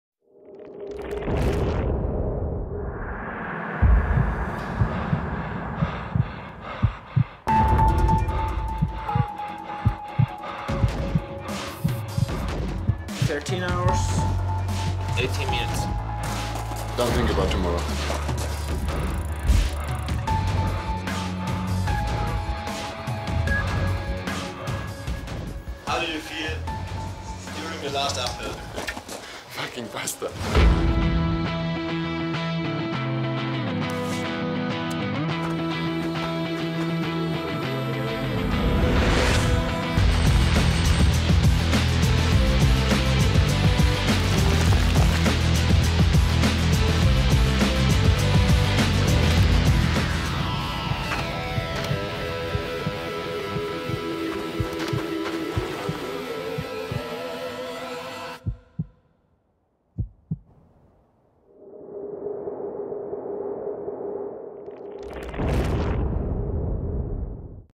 DYNAFIT goes Summer
What is DYNAFIT doing in Summer? Alpine Running and Alpine Biking!